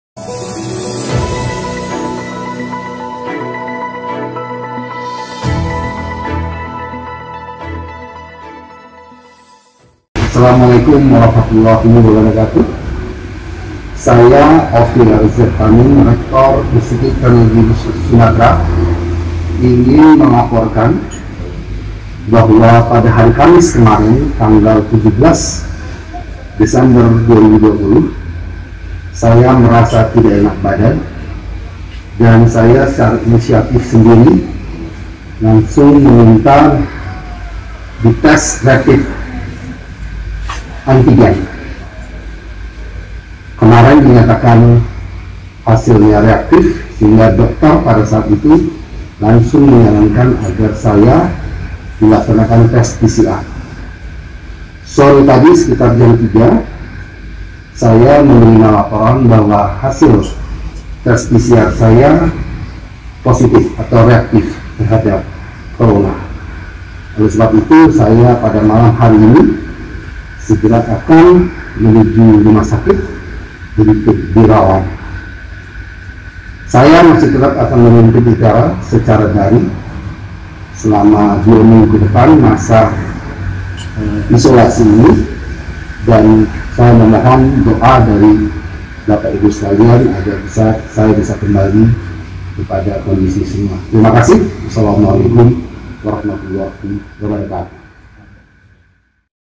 Rektor Itera Prof Ofyar Z Tamin Positif Covid-19
Rektor Institut Teknologi Sumatera (Itera) Prof Ofyar Z Tamin terkonfirmasi positif COVID-19.

Hal ini diketahui dalam unggahan video di Instagram @iteraofficial.  Dalam unggahan video tersebut, Rektor mengatakan bahwa sebelumnya merasakan tidak enak badan.

"Saya ingin menyampaikan bahwa pada Kamis 17 Desember 2020, saya merasa tidak enak badan. Lalu saya berinisiatif meminta dirapid test antigen, dan kemarin hasilnya reaktif," ungkapnya.

Sementara pada saat hasil rapid test reaktif, ia langsung melakukan tes swab. Berdasarkan hasil swab PCR, dirinya dinyatakan positif COVID-19. "Pada Jumat (18/12) sore, saya menerima laporan bahwa hasil PCR saya reaktif atau positif COVID-19," ujarnya.

Dalam video tersebut, Rektor juga menyatakan bahwa dirinya akan segera menuju rumah sakit untuk menjalani isolasi. Meski begitu ia akan tetap bekerja secara online.

"Saya akan segera datang ke rumah sakit untuk dirawat atau menjalani isolasi. Saya tetap akan memimpin Itera secara daring selama masa isolasi ini," katanya.

Rektor juga meminta doa dari semua pihak agar dapat sembuh. "Saya meminta doa dari bapak ibu skalian agar dapat kembali pada kondisi semula," ujarnya.